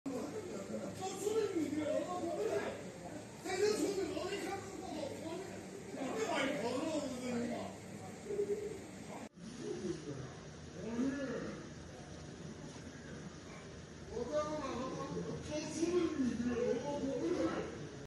Security Guard Flees From Wild Boar in Wuhu, China
CCTV footage caught the moment a wild boar got loose inside a security complex in Wuhu, China — giving one poor guard the fright of his life.

Though the boar temporarily caused a ruckus, it eventually went on its way without further incident or injuries.